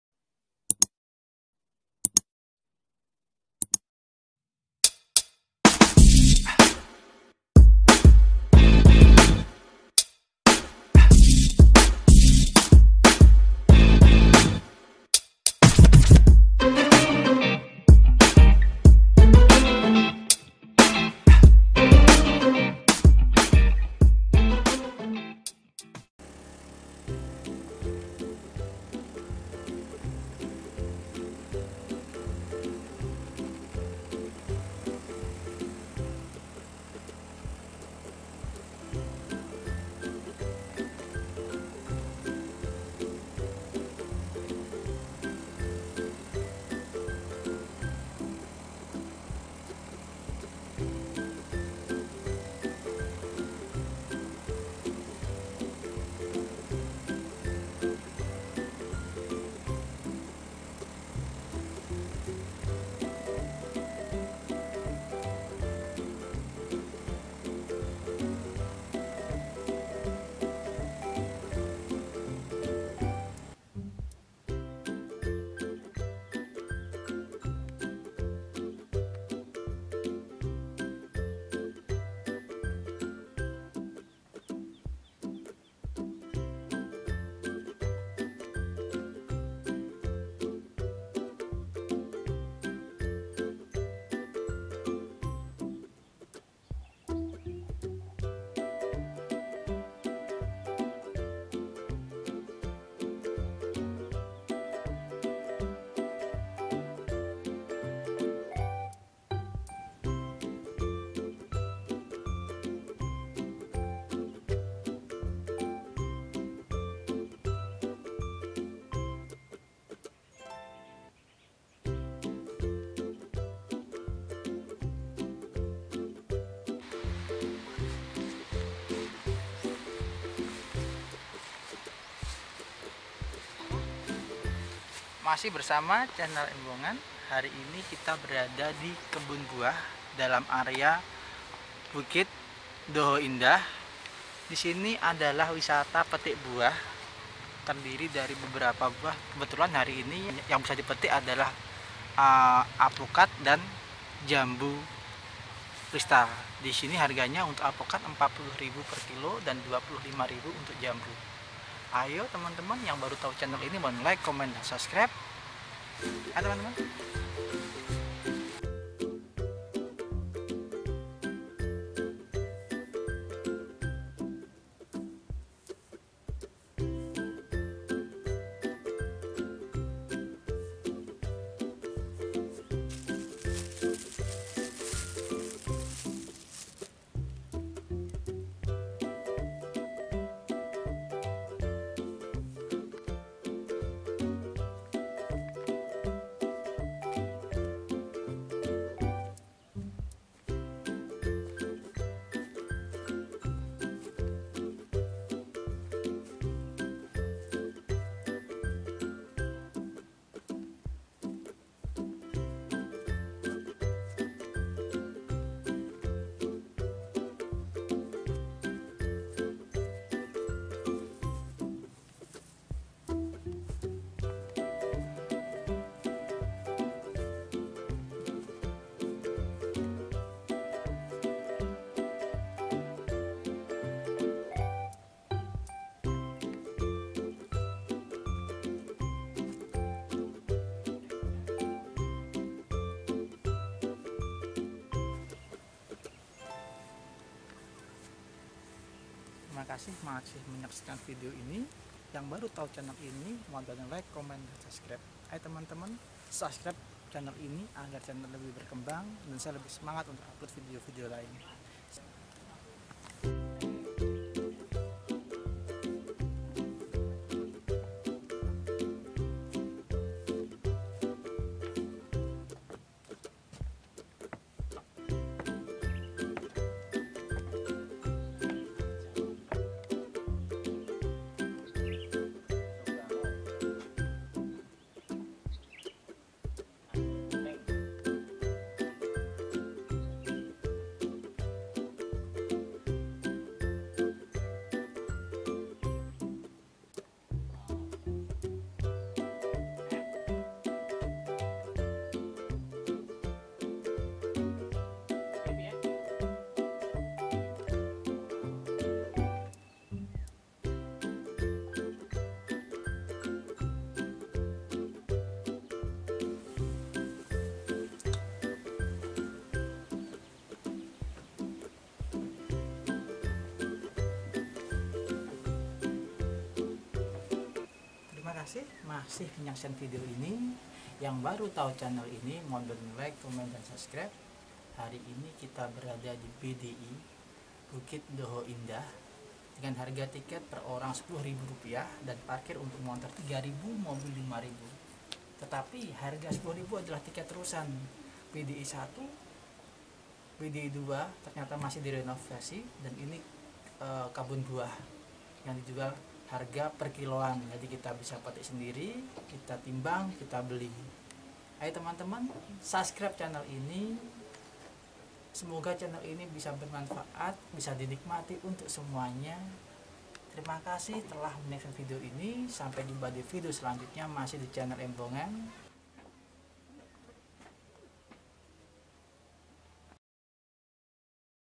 DESTINASI WISATA ALAM EDUKASI TAMAN PETIK BUAH DI BUKIT DHOHO INDAH DI KABUPATEN KEDIRI
Selamat datang di channel embongan atau chaem

Hari ini kita berkunjuk ke Distinasi Wisata Kebun Petik Buah Bukit Dhoho Indah yang terletak di Desa Tiron Kecamatan Banyakan Kabupaten Kediri.

Distinasi Wisata Kebun Petik Buah Bukit Dhoho Indah menawarkan wisata perbukitan alam, serta sarana hiburan yang sangat menarik dan setiap hari terdapat penambahan dan perbaikan dari segi sarana dan prasarana dengan harga tiket masuk Rp. 10.000 untuk tiket terusan

Fasilitas tersedia lengkap seperti wahana buah, kasebo serta kita bisa membeli buah dengan harga Rp. 40.000 per kg untuk Alpukat dan Rp. 25.000 per kg untuk jambu Kristal.

Ayo datang ke sini ajak keluarga dan nikmati Distinasi Wisata Kebun Petik Buah Bukit Dhoho Indah

Salam hormat
Channel embongan